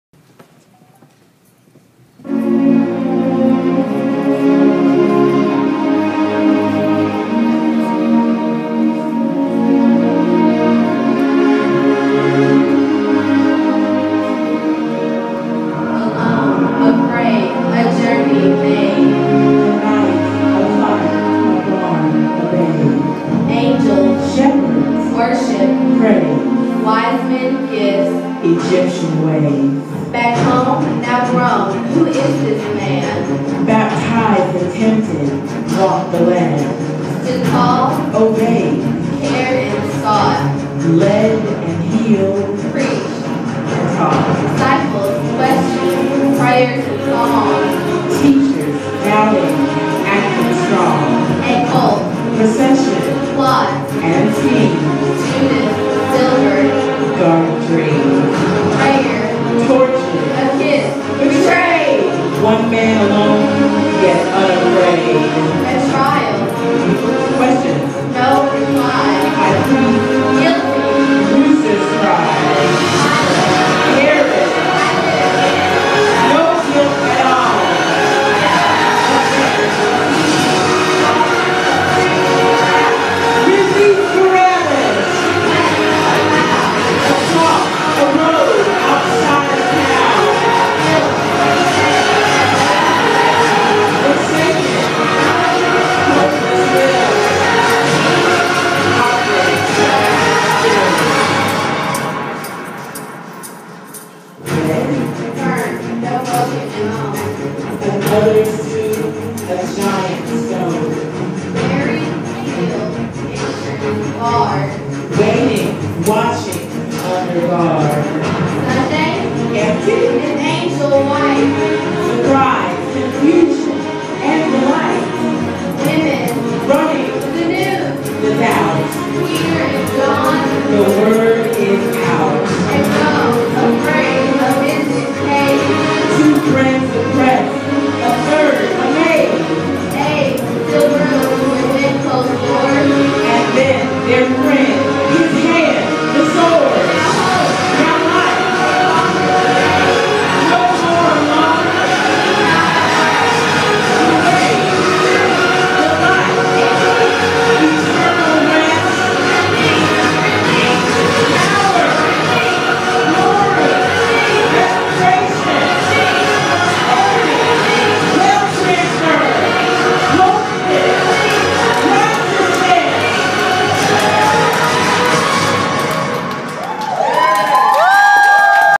JSIKM Prophetic Praise
JSIKM Carissa praises gracefully and beautifully to one of her unique creations.